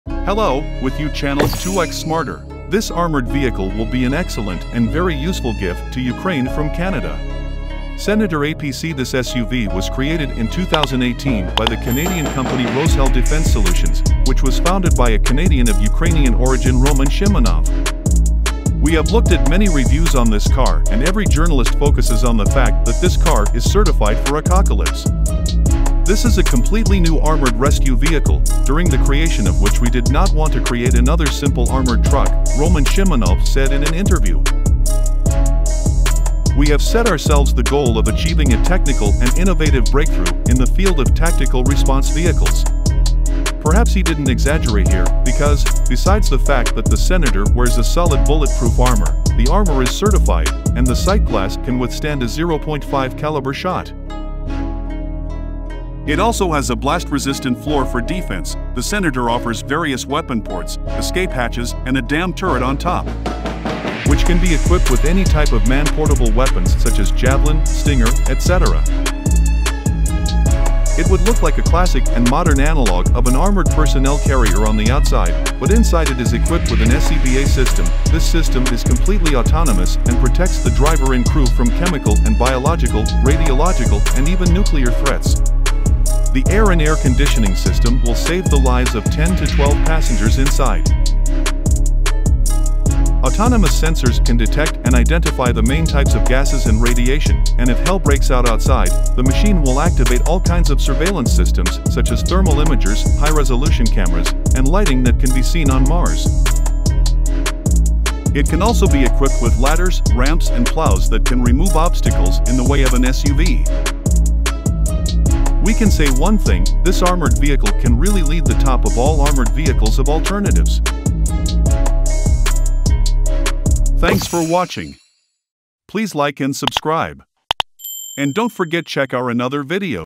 🚘 How Good is Senator APC? Canada Will Send a Large Batch of Senator Armored Vehicles to Ukraine
Canada will send a large batch of Senator all-terrain vehicles to Ukraine

Defense Minister Anita Anand said Ottawa will provide Kyiv with 200 armored SUVs.

We tell you what this car is like.

🇨🇦Destination
The Senator APC armored car is designed to deliver personnel to the battlefield off-road and to patrol in urban areas.

🇨🇦Armor
Steel protection withstands shelling from 7.62 mm machine guns, explosions of grenades and anti-personnel mines under the bottom. The seats are equipped with blast protection devices. The tires can be inflated on the go and are equipped with bulletproof inserts.

🇨🇦 Crew
In addition to the driver, the armored car can carry up to 12 infantrymen. They can conduct surveillance through video cameras and thermal imagers.

🇨🇦Armament
Depending on the tasks, the combat module can be equipped with a 7.62-mm or 12.7-mm machine gun; as well as a 40 mm automatic grenade launcher. The crew can fire without leaving the armored car through the loopholes.

🇨🇦Protection
The armored car is equipped with air filtration systems and protection against chemical, nuclear and biological weapons.

🇨🇦Power plant and speed
Ford 6.7TD V8 engine with 400 hp provides a maximum speed of 110 km / h.

This vehicle excels at transporting infantry to the line of fire in off-road conditions and is ideal for patrolling the streets of ruined cities.
Channels Television
Channels TV
Breaking news
latest news
Ukraine Senator 4x4 APC
Ukraine Senator 4x4 armored
Roshel Senator
russia ukraine
how win russia